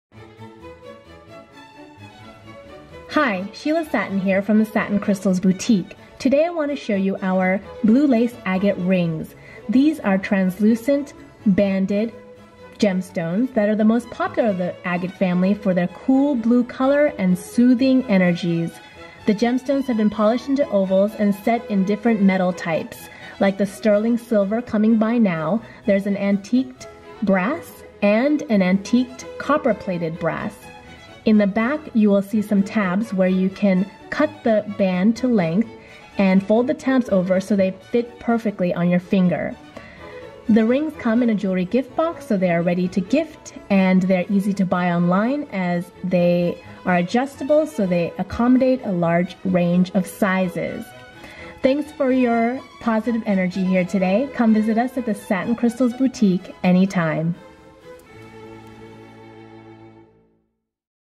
Wear Blue Lace Agate Rings - Satin Crystals Jewelry Shop
Learn the crystal healing properties of Blue Lace Agate and how to wear these beautiful banded stones in adjustable tab rings. Blue Lace Agate rings await you at the Satin Crystals Boutique

Blue Lace Agate rings are cool and calming. You feel refreshed wearing these gems. The handmade rings are available in adjustable Sterling Silver, Copper, and Brass tones. Each piece is made in our California studio by Sheila Satin.

These are many other stone rings available at the Satin Crystals Boutique

Thank you for your positive presence. We appreciate likes, comments, subscribes, and shares!

Sincerely,